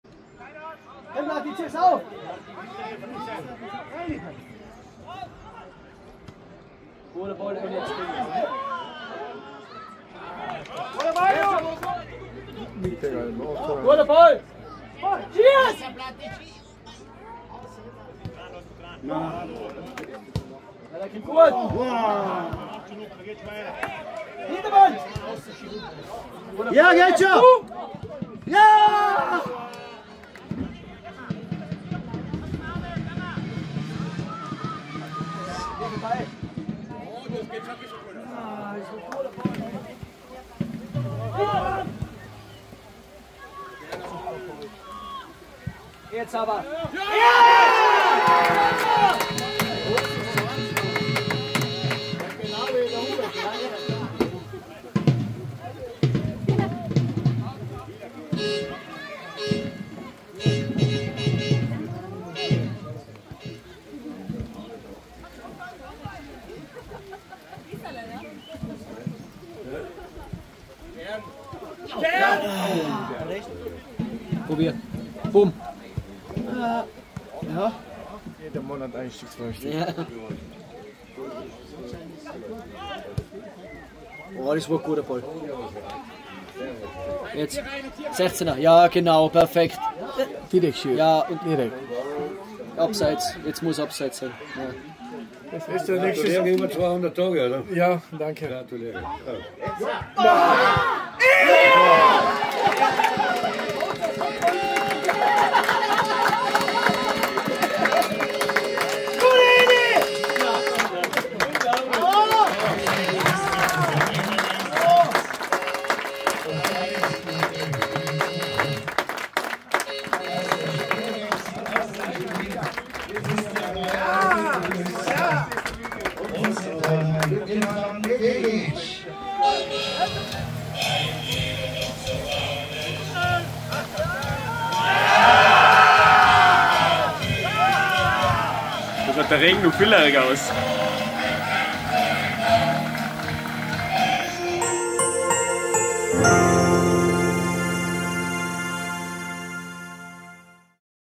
SV Stans 2-0 FC Wildschönau (Bezirksliga Ost Tirol)
Tore: Florian Essl, Elias Schwarz